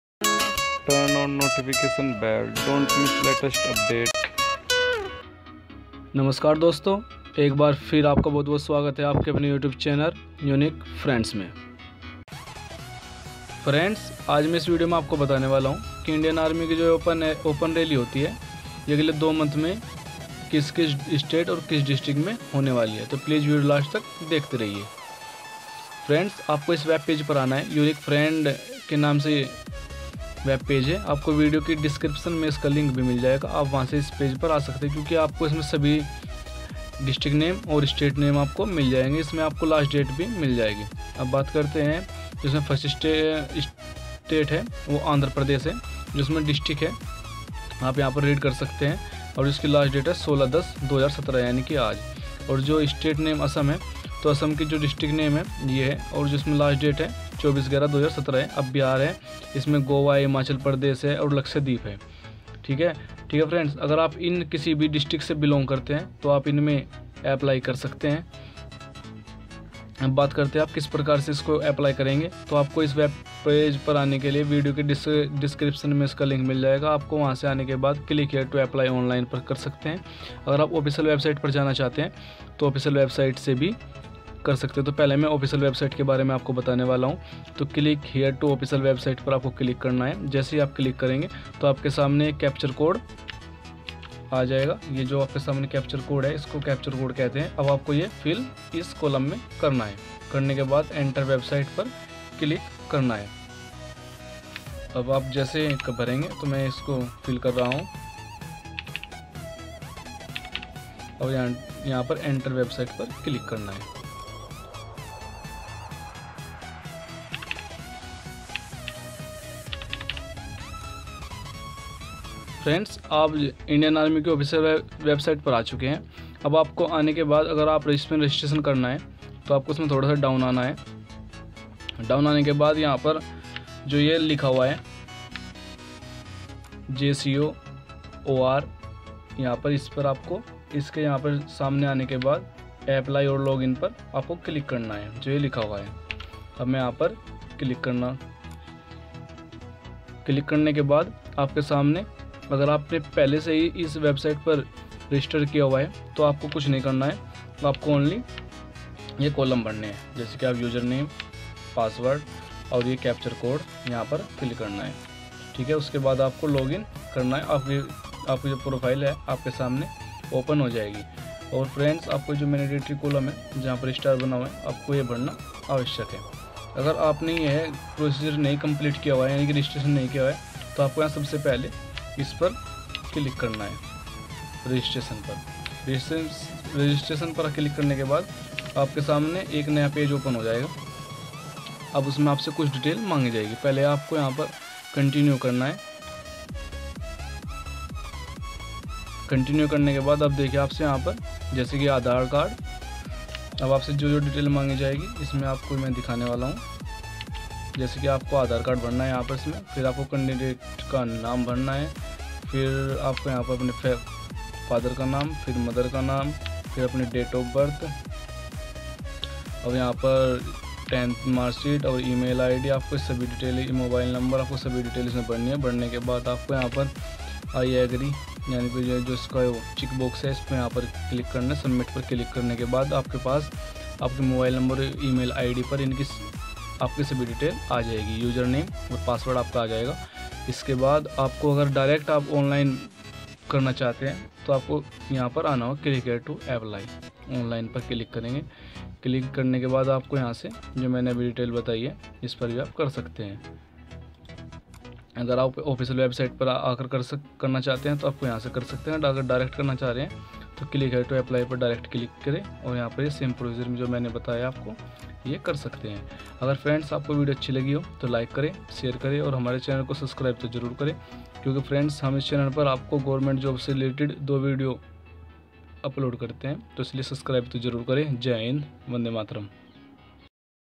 INDIAN ARMY NEXT TWO MONTH OPEN RALLY 2017
नमस्कार दोस्तों..   आज के इस विडियो में मैंने बताया है कि अगले दो महीने में आने वाले INDIAN ARMY की रैली भर्ती 2017 के बारे में जानकारी दी है

नमस्कार दोस्तों, आपका बहुत स्वागत है आपके अपने YouTube चैनल 'Unique Friends' में

धन्यवाद!!

unique Friends
"Uniquefriends"
Gaurav Kumar